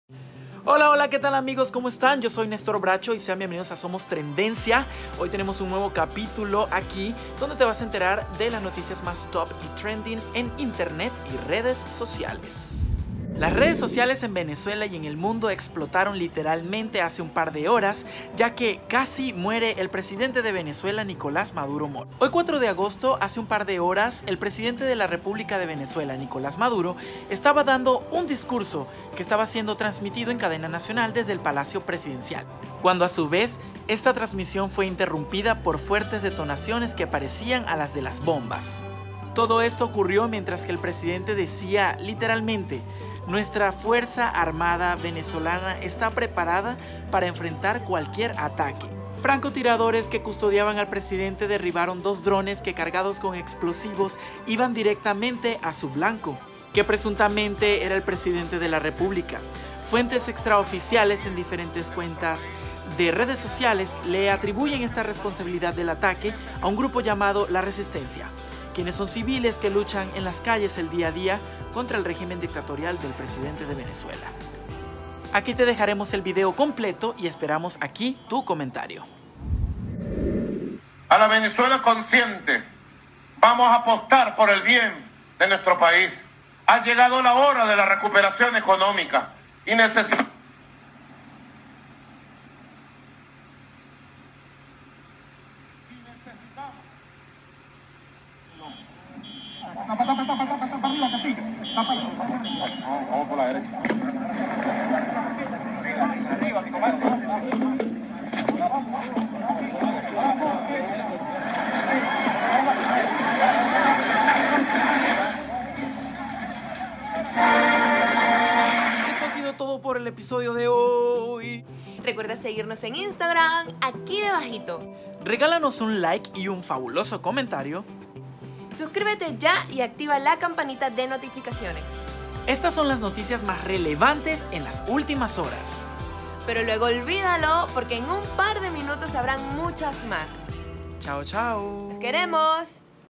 Intento de Asesinato a Nicolás Maduro Vídeo Completo
Casi consiguen asesinar al presidente de Venezuela
Revisa los detalles en Laneta.com